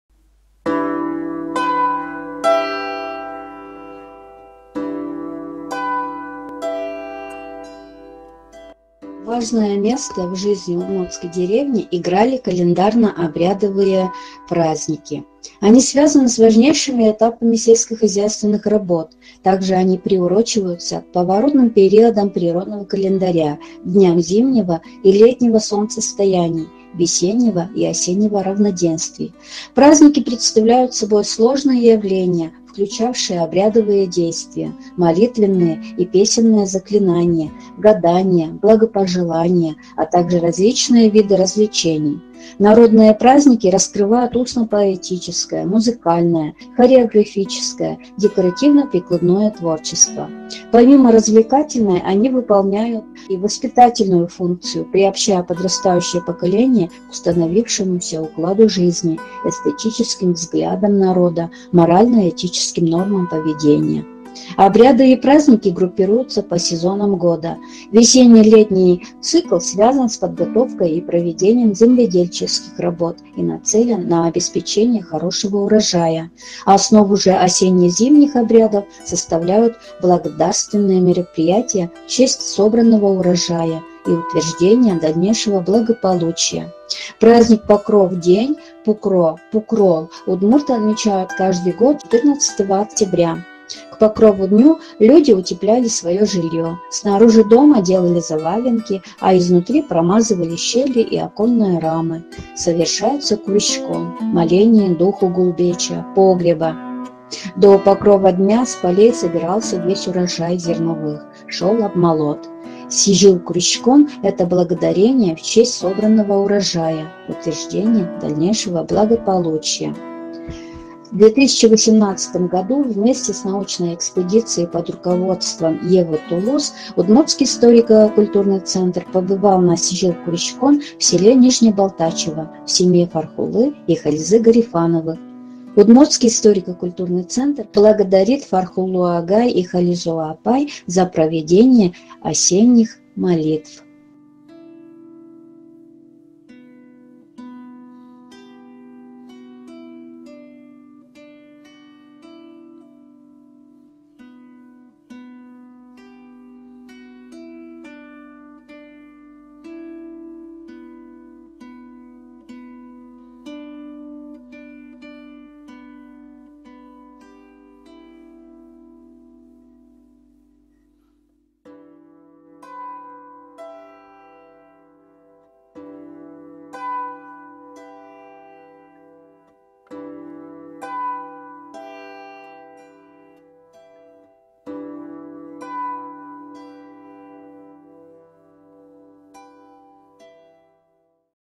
Удмуртский обряд Cизьыл куриськон
Важное место в жизни удмуртской деревни играли календарно-обрядовые праздники. Они связаны с важнейшими этапами сельскохозяйственных работ. Также они приурочиваются к поворотным периодам природного календаря: к дням зимнего и летнего солнцестояний, весеннего и осеннего равноденствий. Праздник Покров день – Пукро (Пукрол) удмурты отмечают каждый год 14 октября. К Покрову дню люди утепляли свое жилье. Снаружи дома делали завалинки, а изнутри промазывали щели и оконные рамы. Совершается куриськон – моление духу гулбеча (погреба).
До Покрова дня с полей собирался весь урожай зерновых, шел обмолот. Сизьыл куриськон -это благодарение в честь собранного урожая, утверждение дальнейшего благополучия.
Видеоролик подготовлен Удмуртским историко-культурным центром.